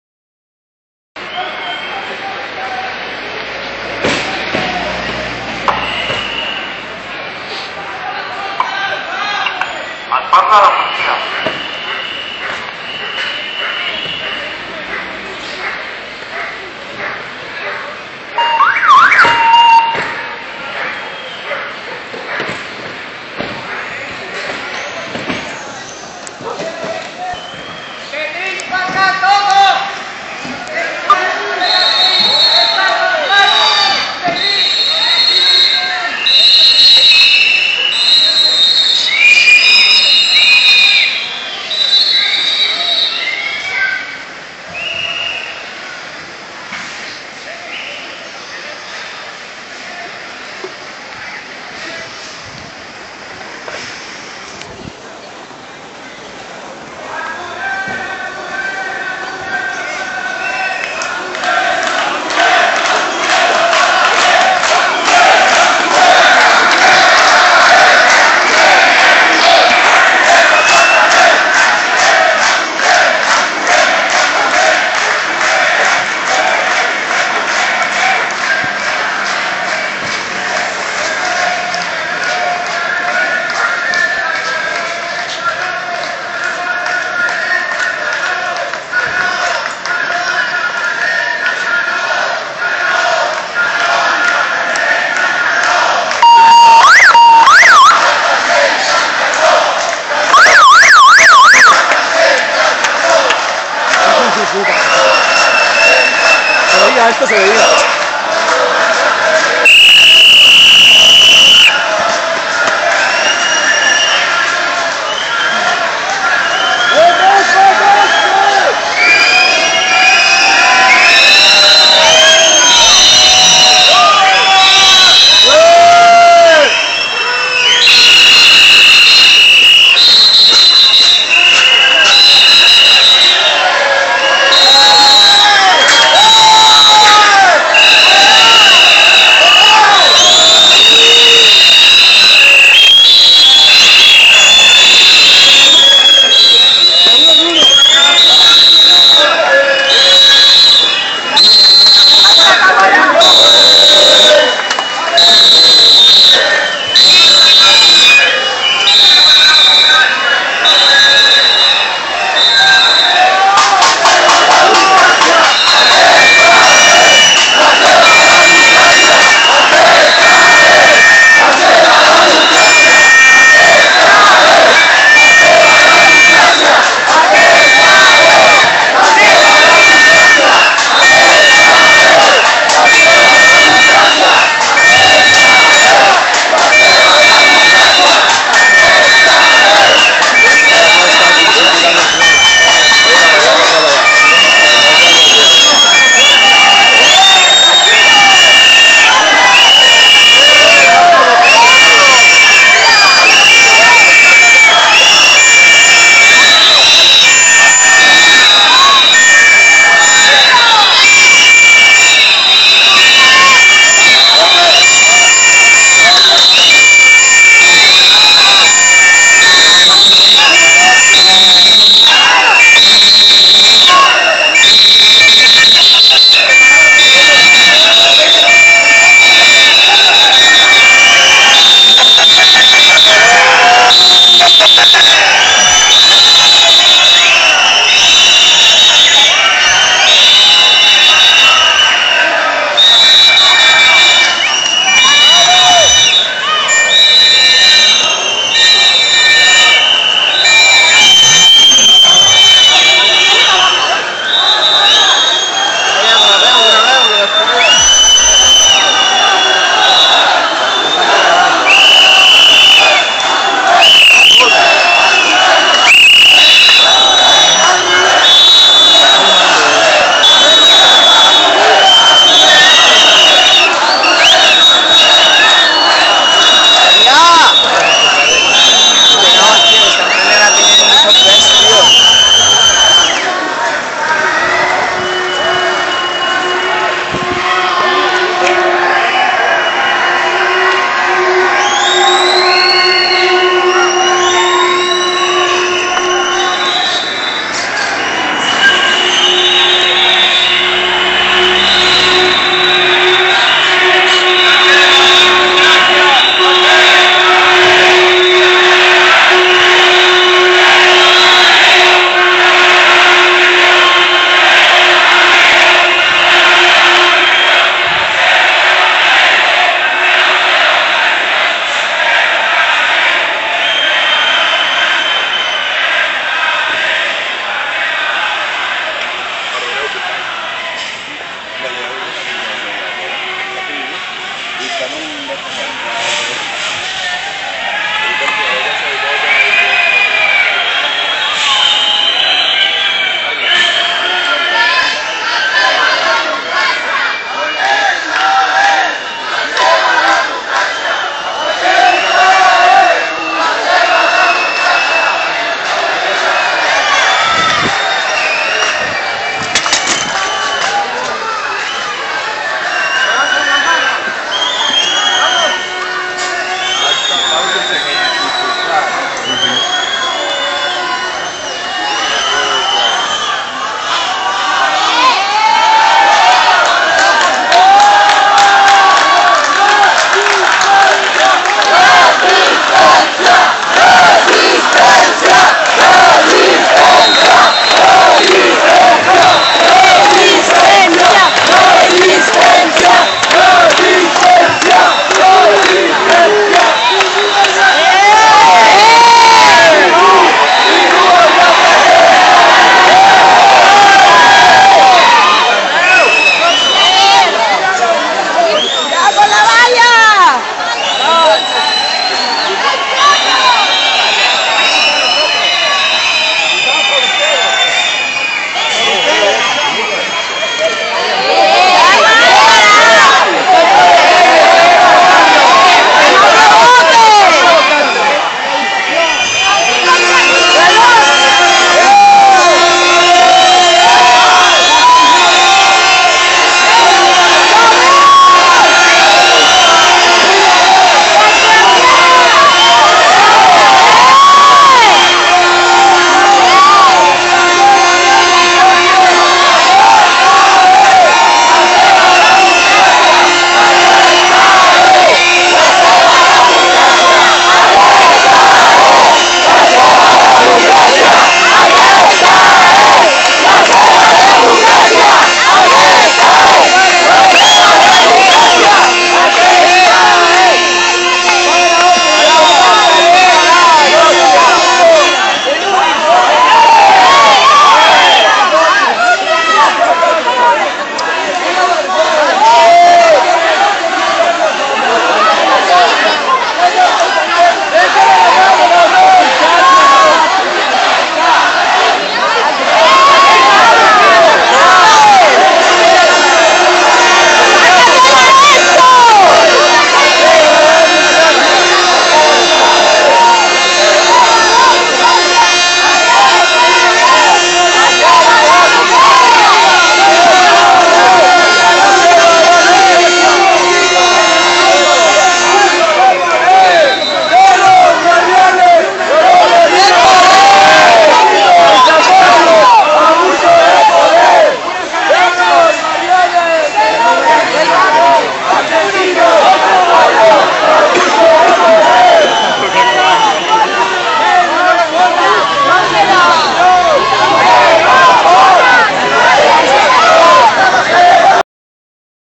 Desallotjament de l'entrada al parc
Arribada de les furgones dels mossos i desallotjament (neteja?) de l'entrada Pujades/Wellington al Parc de la Ciutadella.